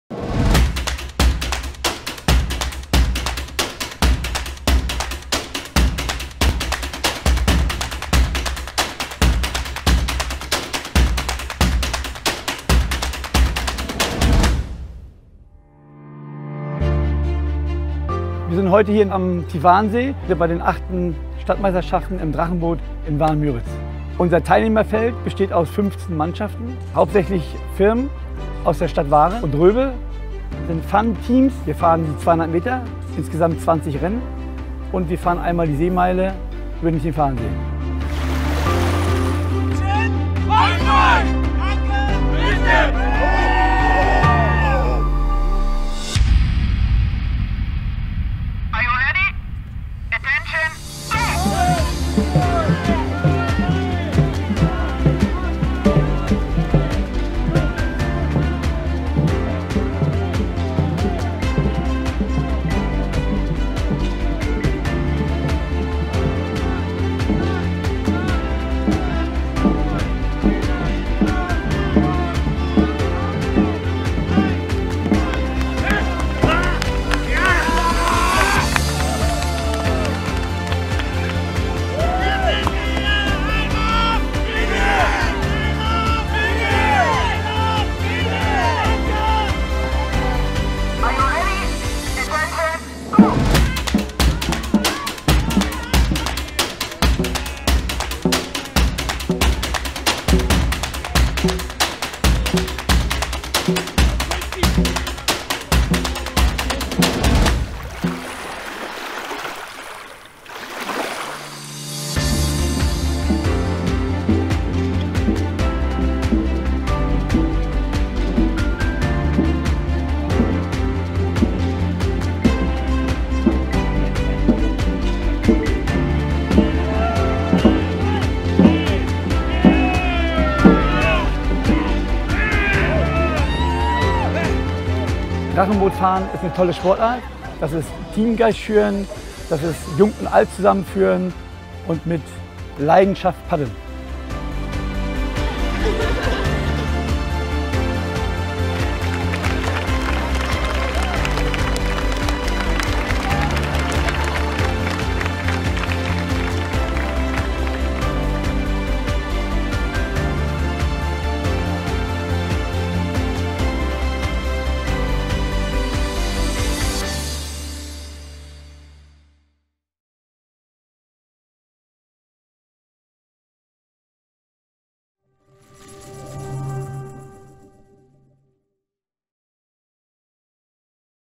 Stadtmeisterschaft Drachenboot 2018 - Waren Müritz
Der Imagefilm zur Stadtmeisterschaft 2018 im Drachenboot in Waren Müritz - Mecklenburg Vorpommern

Eine Produktion von Film-Schnitt-Fotografie